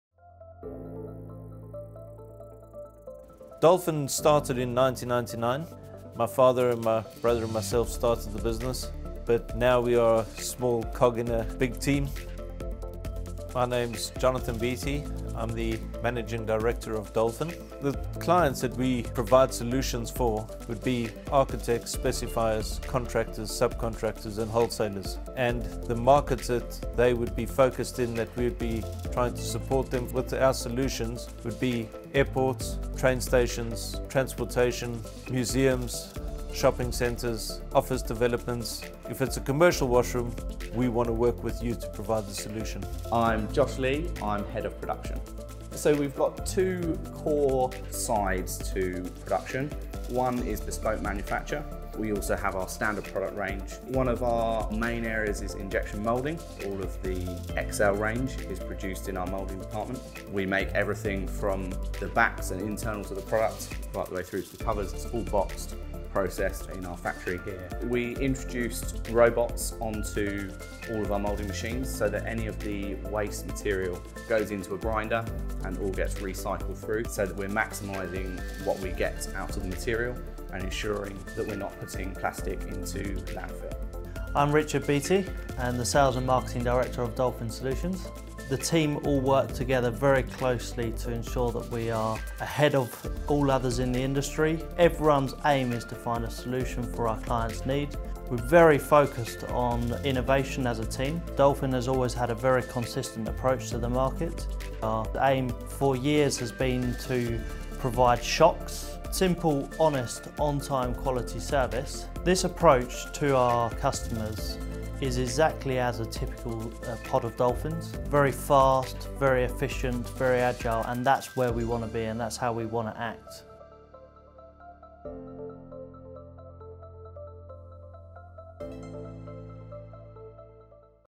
Dolphin Solutions - Corporate Video
BEAUTIFULLY DESIGNED. SUSTAINABLY MADE.

We are Dolphin Solutions, a happy pod of people dedicated to creating the finest commercial washrooms.

Listening carefully, we understand our customers’ needs and design washrooms with the utmost care, integrity, and enthusiasm, creating beautiful, comfortable, high-quality facilities that are intuitively simple to use.

Our approach to service and sustainability aims to support you in improving both washroom hygiene and user well-being, whilst technologies such as our own InForm SMART System help you reduce water and energy consumption and meet environmental targets.

Or Visit our Learning Hub